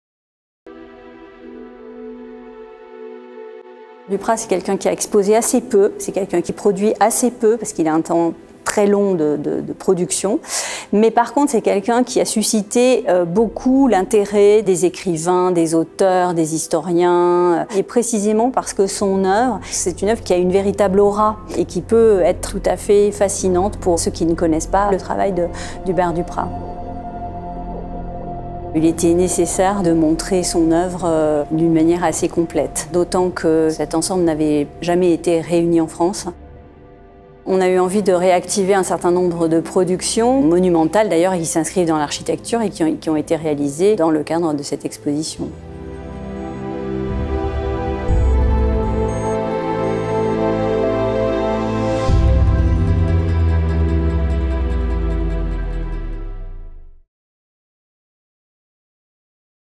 1 minute 1 expo : Hubert Duprat
Vous avez une minute ?

Dans cette mini-série, les commissaires des trois expositions temporaires du MAM Paris vous éclairent sur ces trois artistes à ne pas manquer.
Découvrez l’exposition Hubert Duprat avec Jessica Castex !